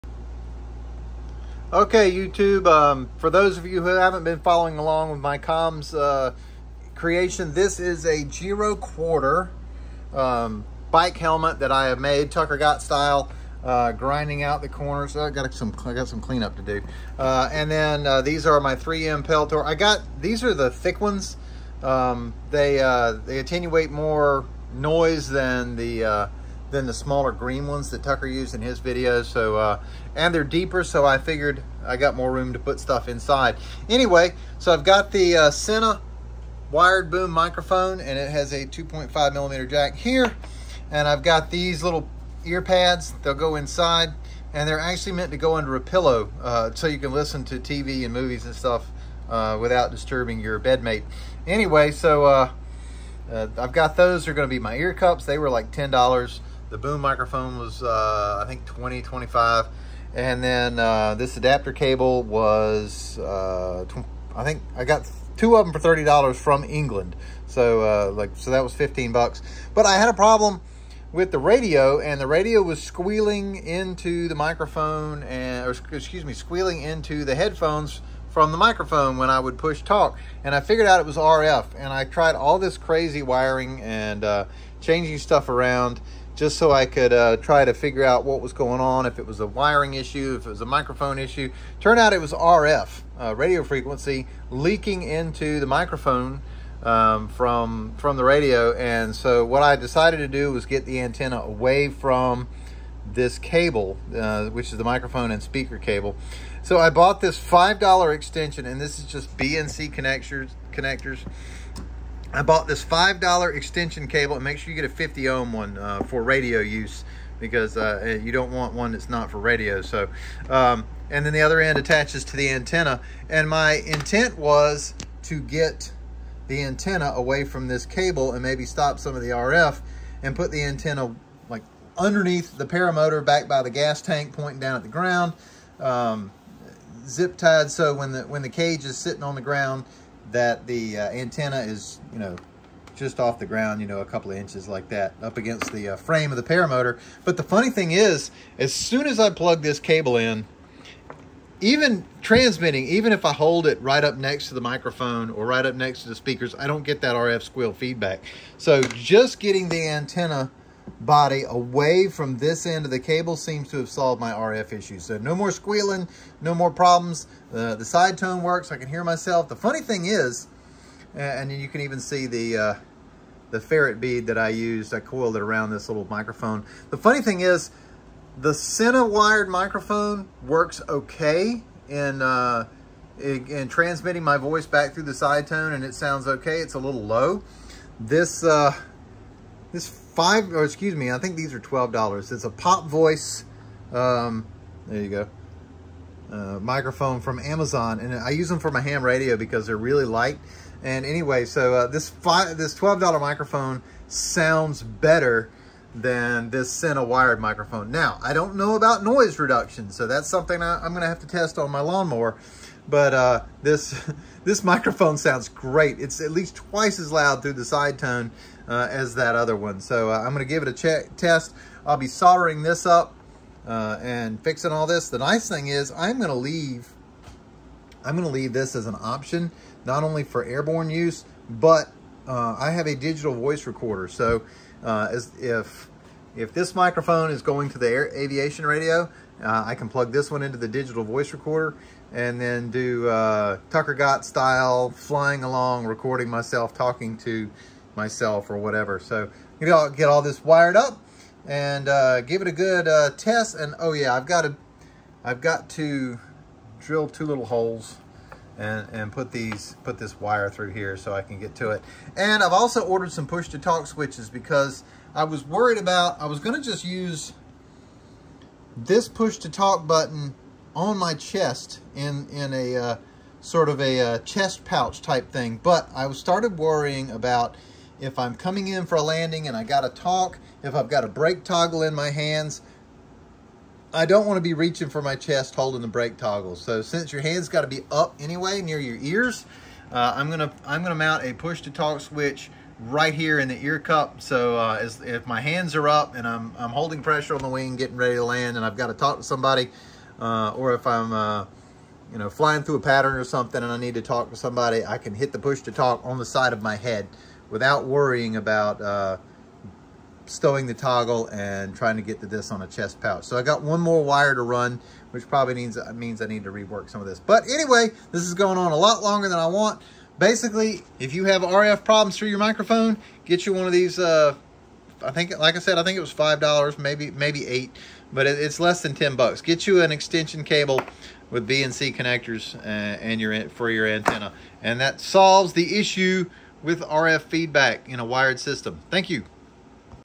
Comms helmet RF Squeal Fixed!
A little RF issue on my @TuckerGott inspired Giro flight helmet. Solved the microphone feedback squeal. Just have to do a little soldering and drill a couple of holes. Also thanks to @IrisPPG I'll be ordering my Iris Infinity first of next week. I look forward to doing a review of it.